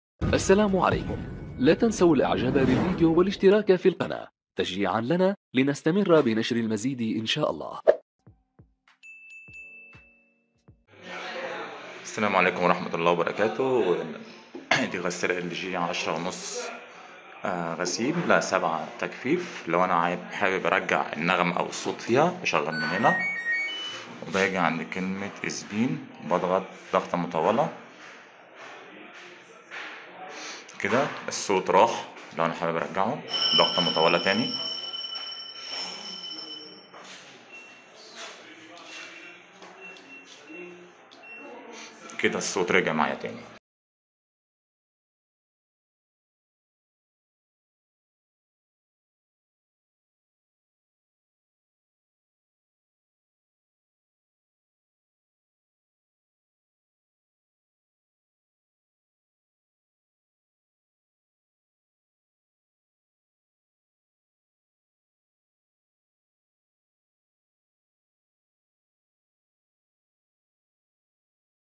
استعادة صوت النغمة التنبيه غسالة ال جى 10.5 كيلو او 9 كيلو بمجفف دراى
طريقة استعادة الصوت فى غسالة lg
ال جى 10.5 كيلو 7 مجفف

أو دعم القناة بالانتساب فيها عن طريق اشتراك شهرى